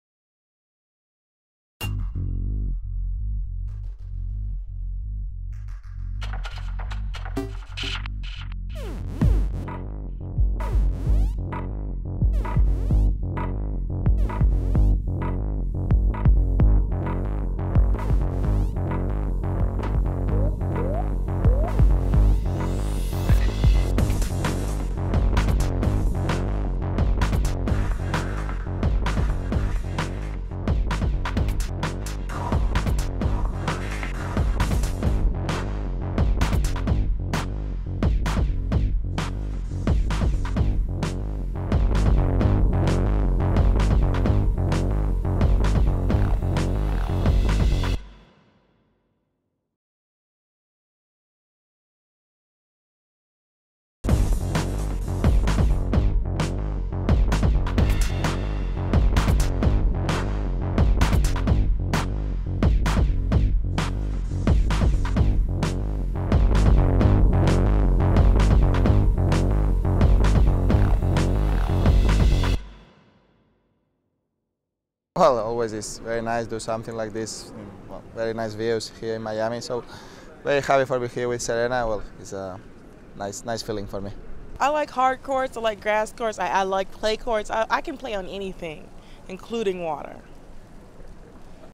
Sony Ericsson 'Walking on Water'
To celebrate the start of the 2008 Sony Ericsson Open in Miami, Serena Williams and Rafael Nadal featured in the world's first tennis match to be played on water